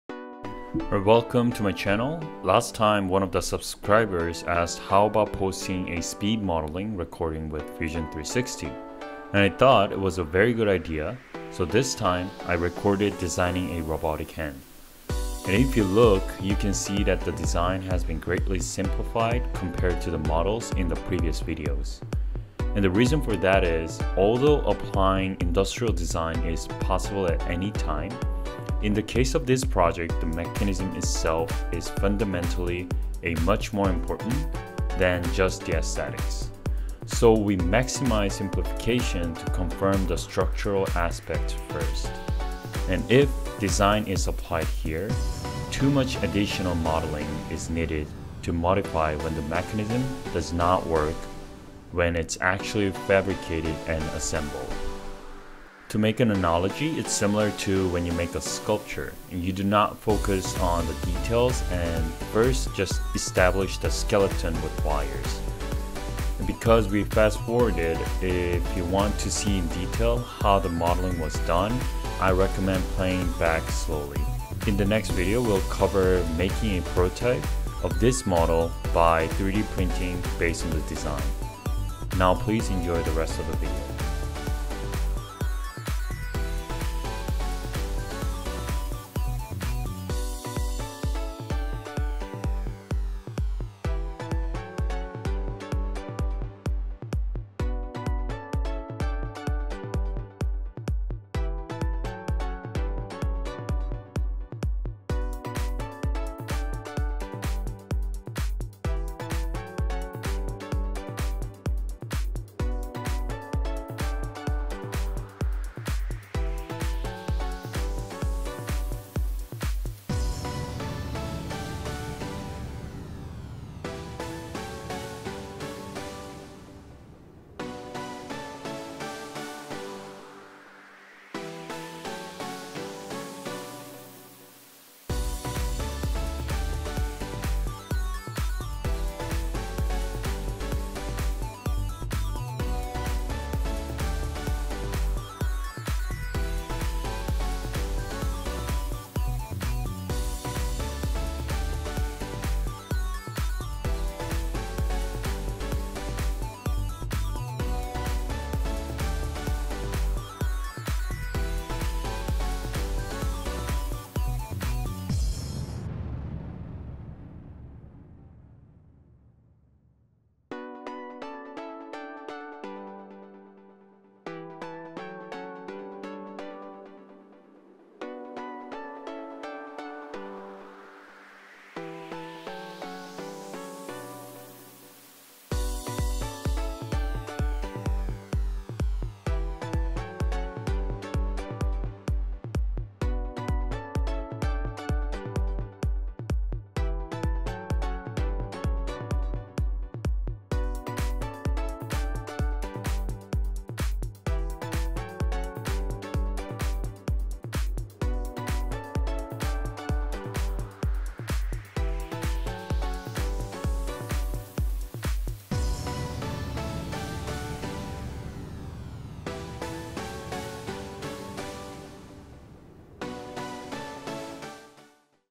Robotic hand with Fusion 360 (Modeling timelapse)
So, we decided to take a step back  to the simple design for now (Almost like a skeleton shape) 👣.
Before adding aesthetic details to this 4-linkage-system, we wanted to make sure everything is working fine.

This was a recording of that modeling process, and we hope to share the making process in the next video.